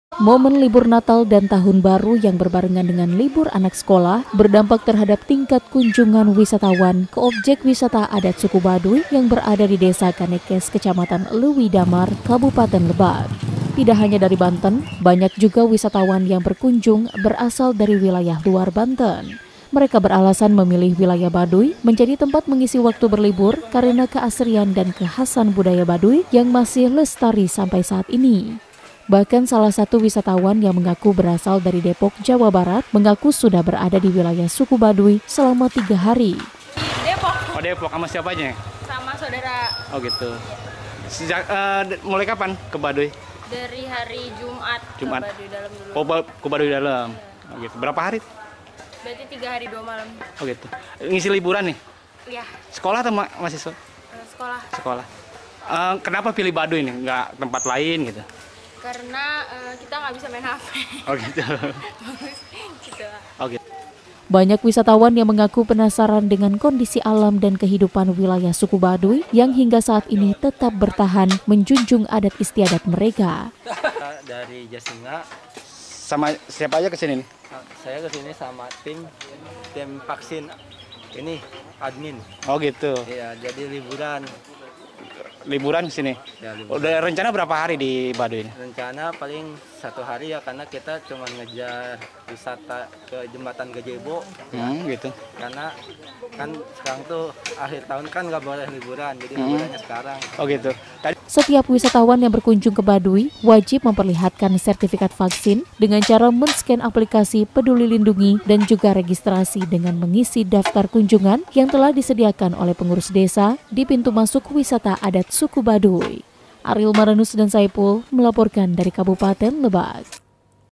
LIBUR AKHIR TAHUN, WISATA ADAT SUKU BADUY RAMAI DIKUNJUNGI WISATAWAN
LIBUR NATAL DAN TAHUN BARU YANG BERSAMAAN DENGAN LIBUR SEKOLAH OBJEK WISATA ADAT SUKU BADUY YANG BERADA DI DESA KANEKES, KECAMATAN LEUWIDAMAR, LEBAK, BANTEN, RAMAI DIKUNJUNGI OLEH PARA WISATAWAN, BAIK DARI WILAYAH BANTEN MAUPUN DARI LUAR BANTEN.

 PARA WISATAWAN INI SENGAJA DATANG KE BADUY UNTUK MENIKMATI KEASRIAN DAN KEKHASAN BUDAYA SUKU BADUY YANG MASIH LESTARI SAMPAI SAAT INI.